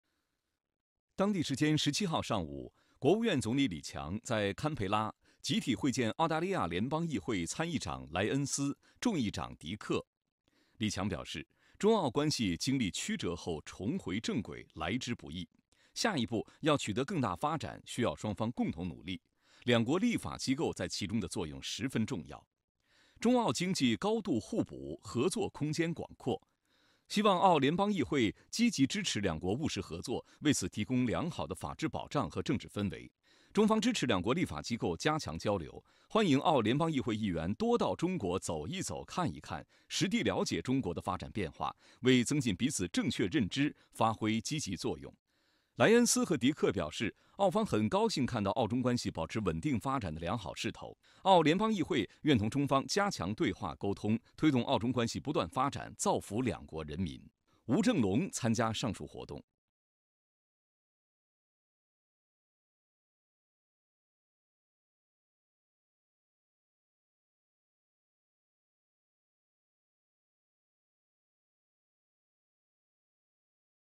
Chinese Premier Says Greater Development of China-Australia Ties Needs Joint Efforts
Chinese Premier Li Qiang on Monday said that a greater development for China-Australia ties requires joint efforts from both sides.